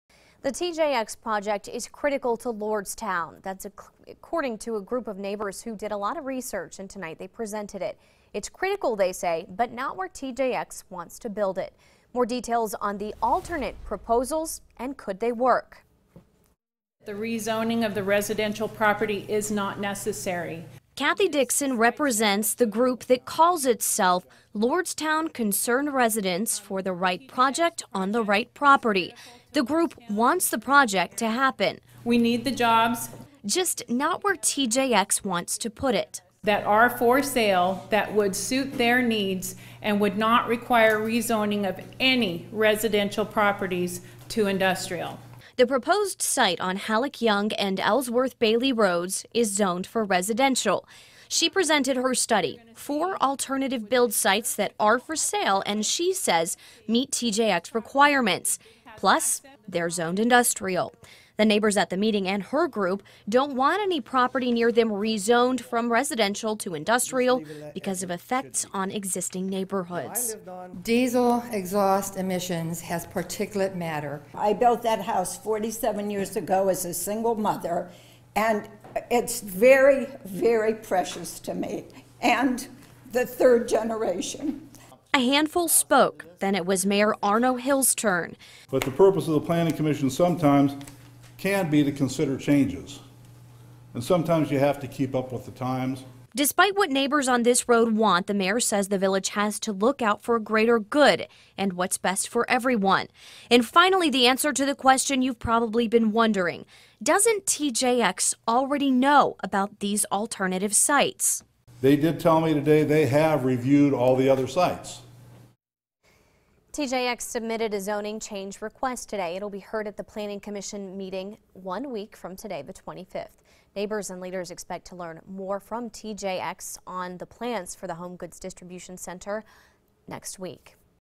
Lordstown residents present alternative sites for proposed TJX facility
The group wants the project to happen -- just not where TJX wants to put it.